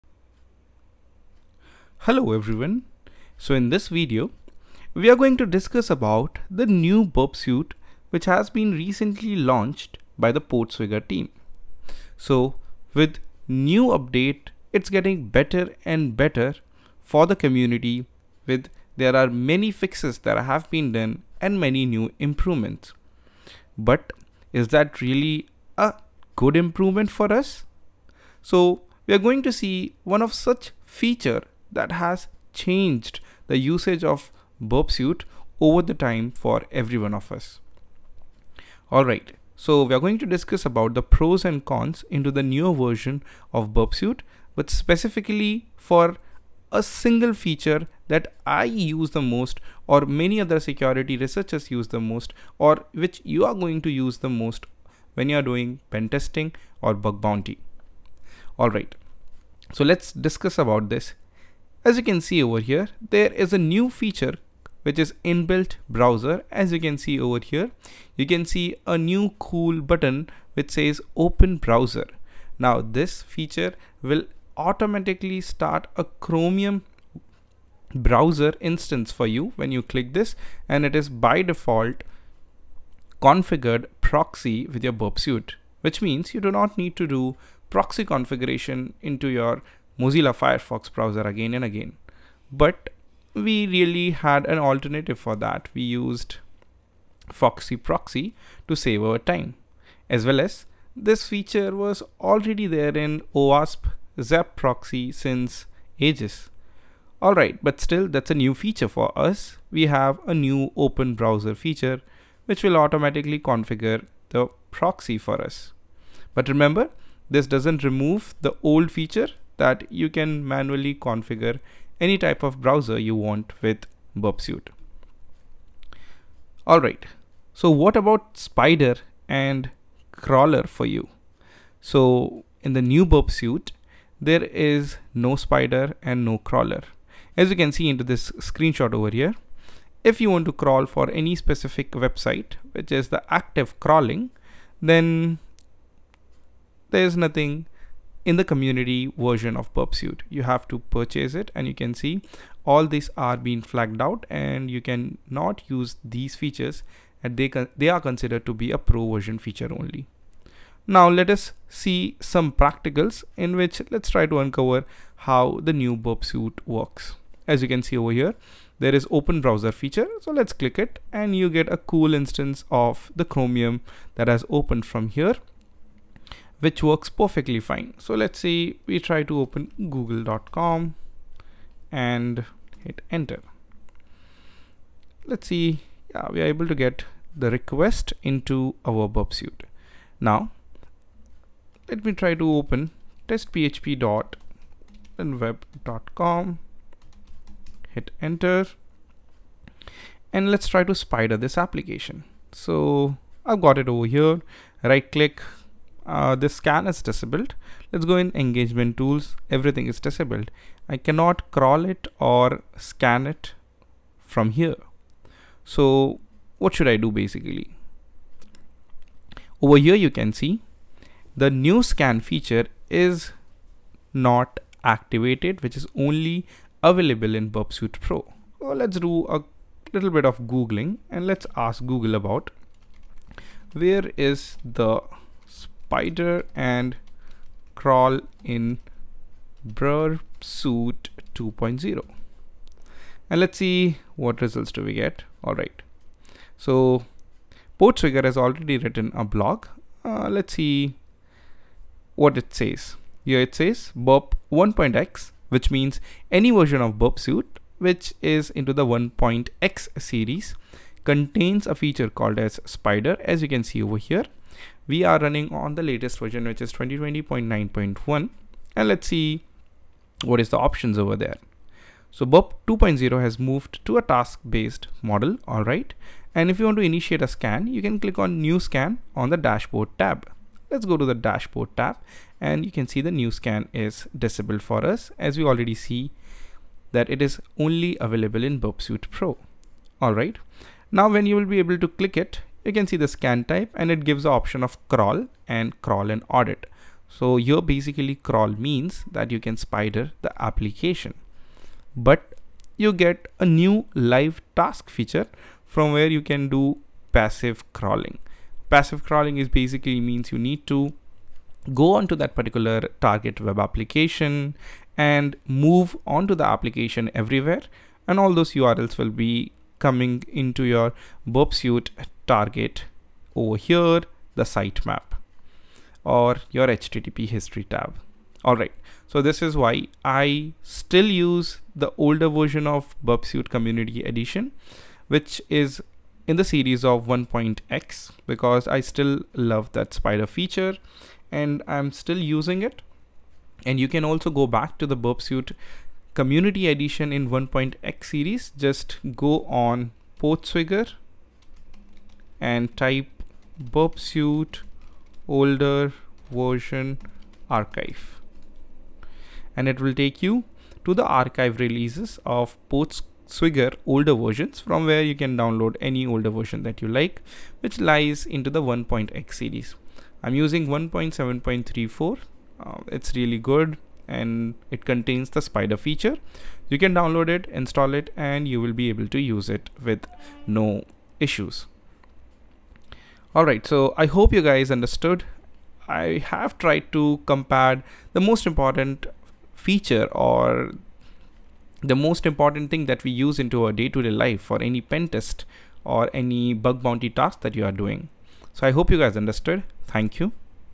Burp Suite 2.0 Poor-man's spider and scanner - Part 1
In this Video, you will learn about Burpsuite 2020 v2.x for Ethical Hacking, Penetration Testing & Bug Bounty Hunting which can be further used for increasing scope.
You will learn attacks principle, methodologies, approach and how to use tools for security testing.

Mindmap - hacktify.in/mindmap.png

Join Telegram Group - bit.do/ytlivebb